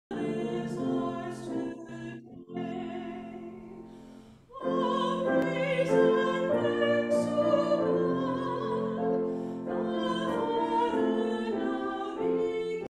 Gellman Room Concert: Sigma Alpha Iota, An International Music Fraternity
The Richmond Alumnae Chapter of Sigma Alpha Iota, an international music fraternity, presents a mix of classical and popular selections featuring pianists, vocalists, and other instrumentalists.